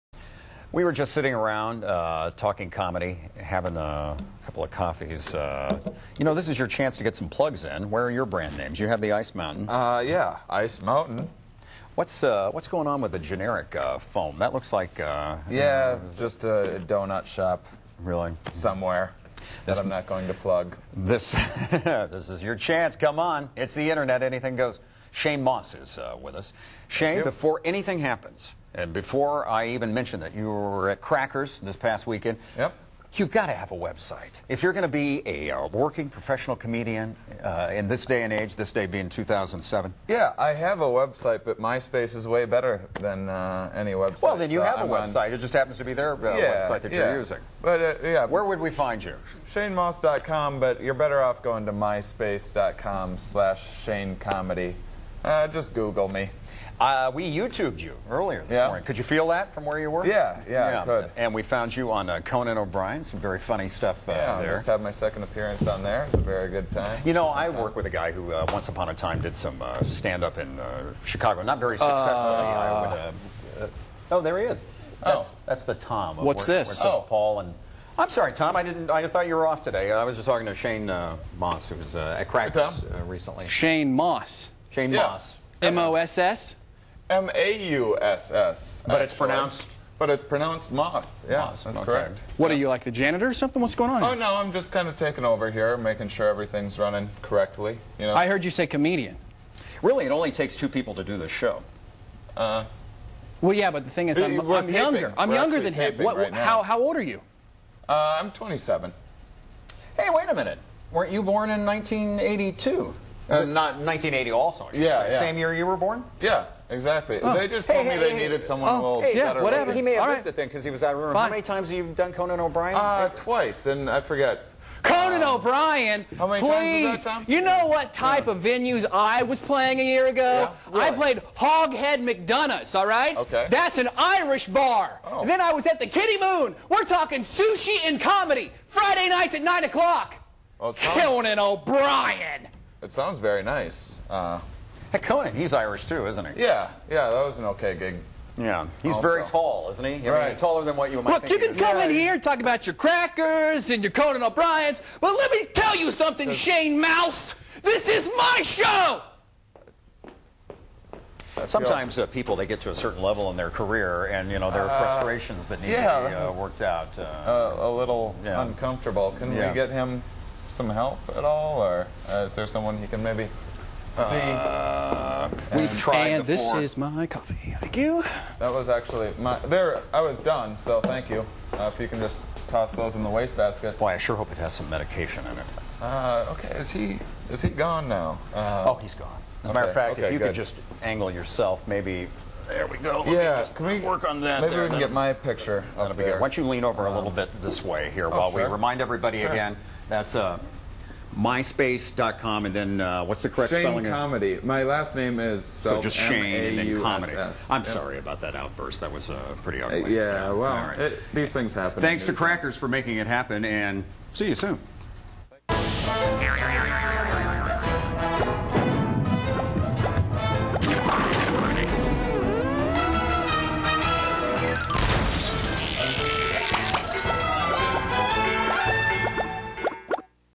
Off The Cuff: Comedian Shane Maus
Paul & Tom go Off The Cuff with comedian Shane Maus.